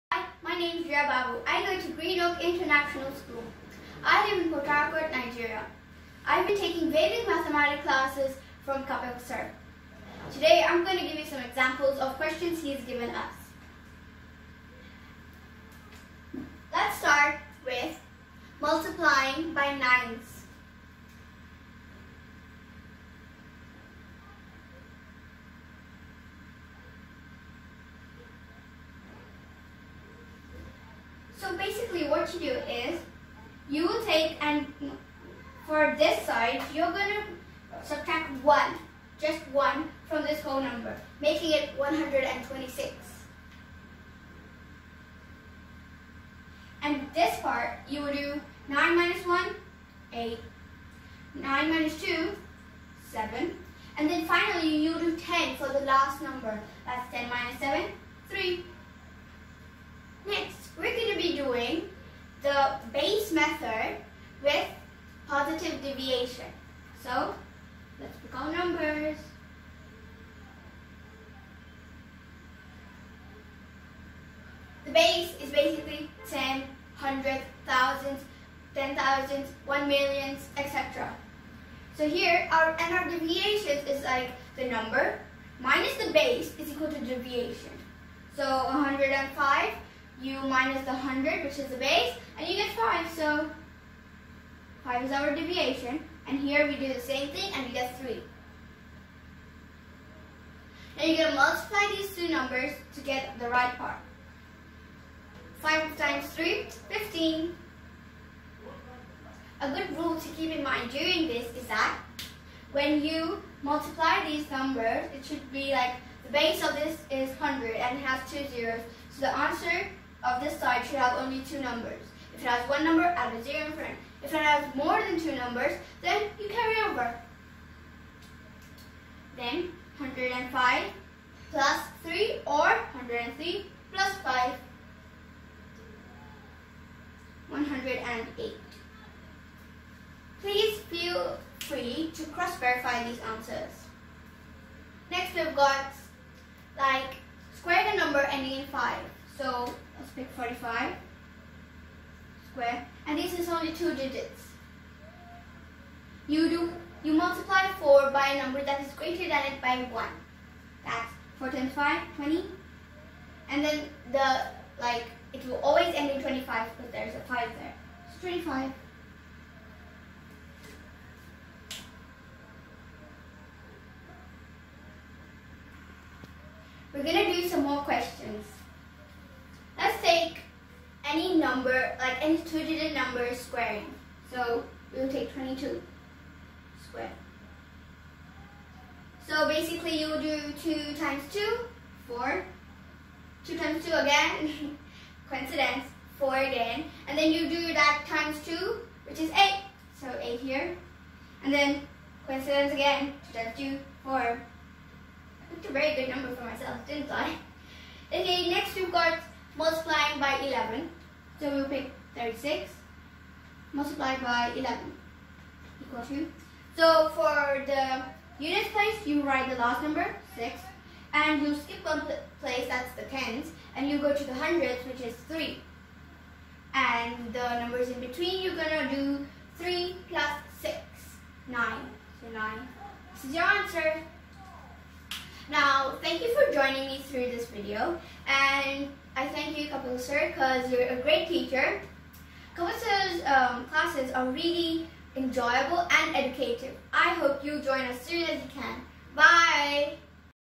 DREA'S TESTIMONIAL FOR ONLINE VEDIC MATHS CLASSES.
THIS VIDEO IS ABOUT THE VIEWS OF DREA FROM NIGERIA ABOUT ONLINE VEDIC MATHS CLASSES FROM KAPIL SIR.